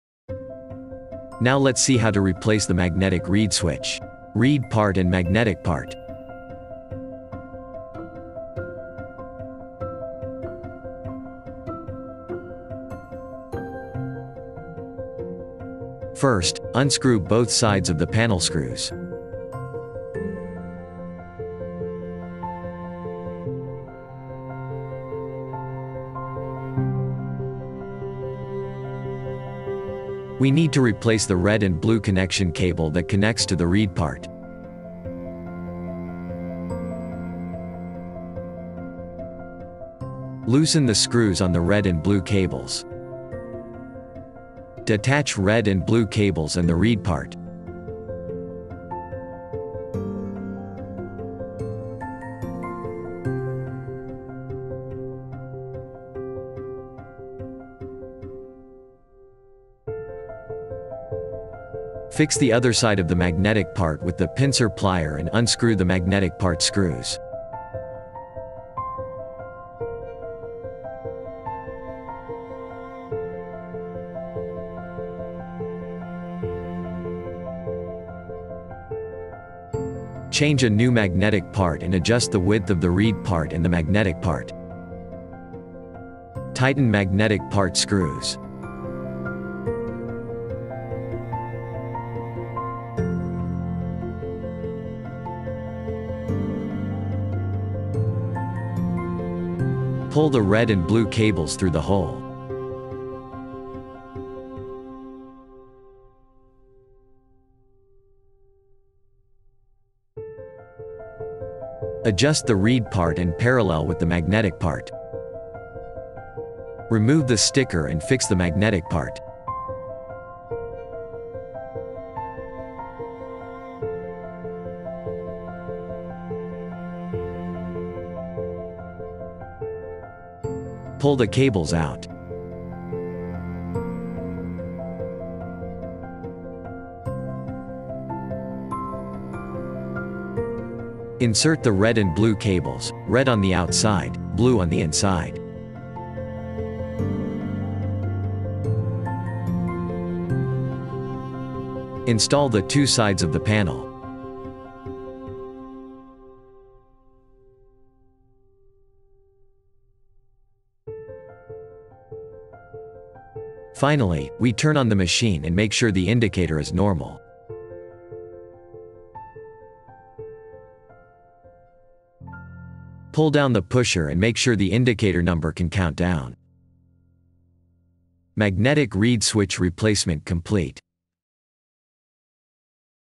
How to change the magnetic switch for Craft Express Plus Tumbler Heat Press Machine?
In this video, we gonna show you how to replace the magnetic reed switch. This video will teach you how to complete a tutorial on how to replace the magnetic reed switch in a short time. When the countdown function fails, you can try it. If you have any questions about the process or want to order any of them.